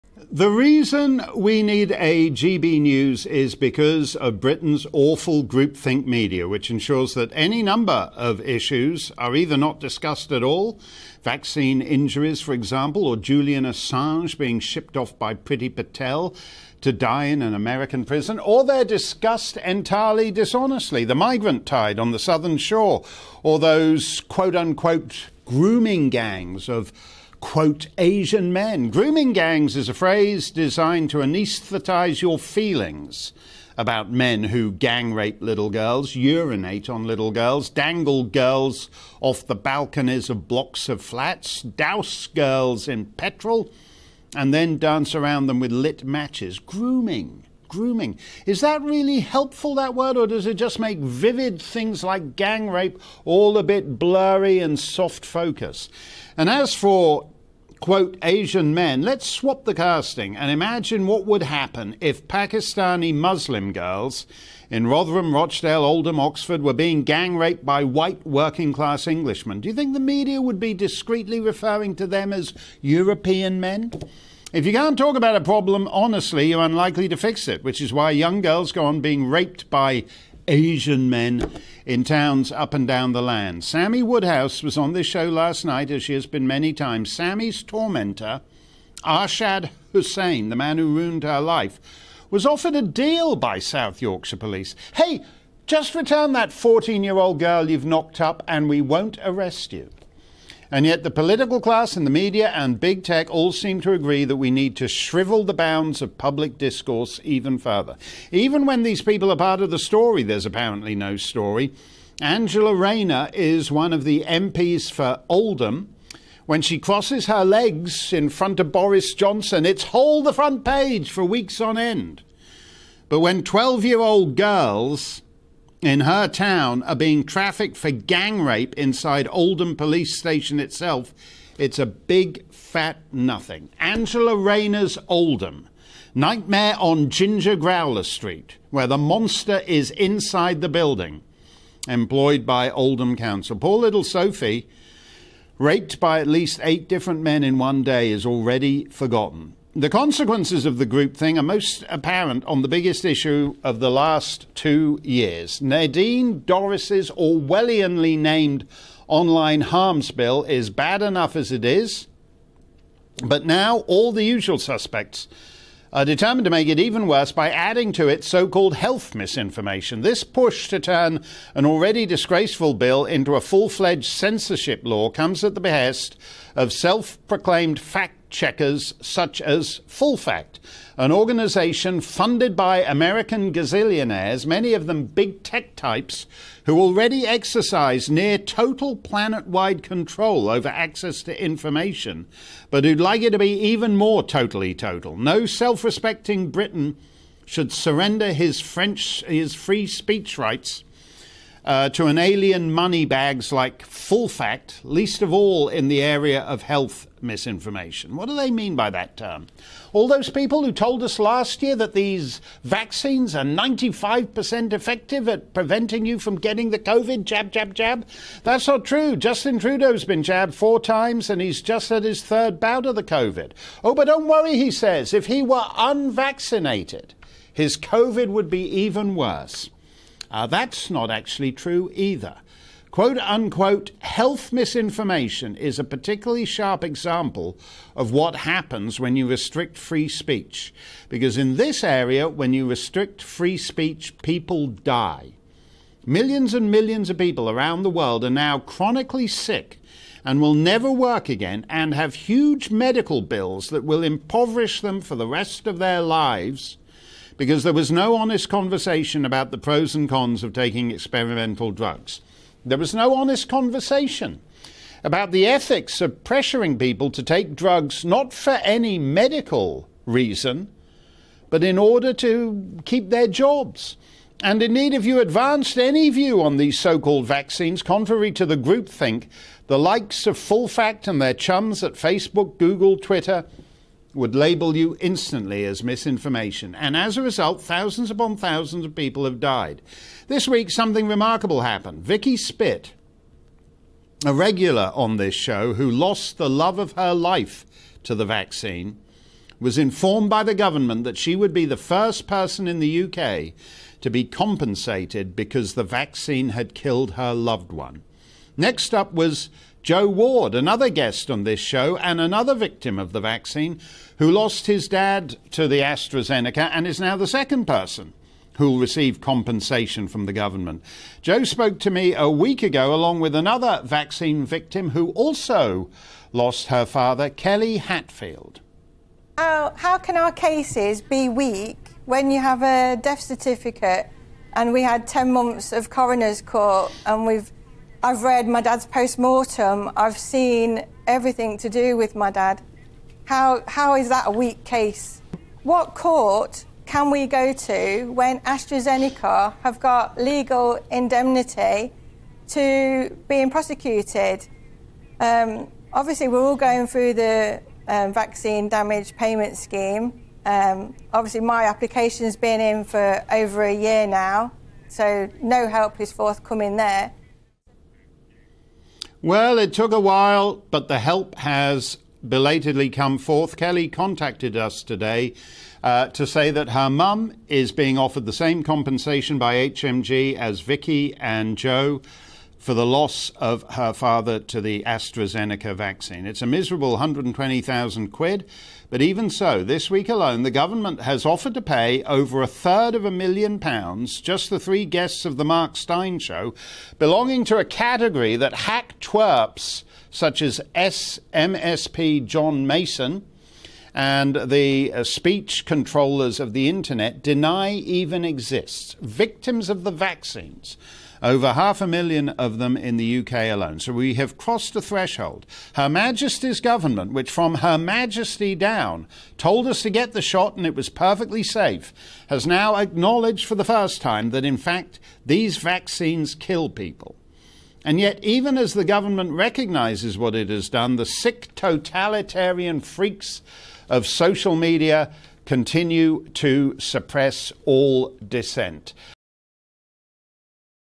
Mark Steyn: 'The sick totalitarian freaks of social media continue to suppress all dissent'
'If you can’t talk about a problem honestly, you’re unlikely to fix it... Yet the sick totalitarian freaks of social media continue to suppress all dissent.'

Mark Steyn takes on the fact-checkers and online censorship.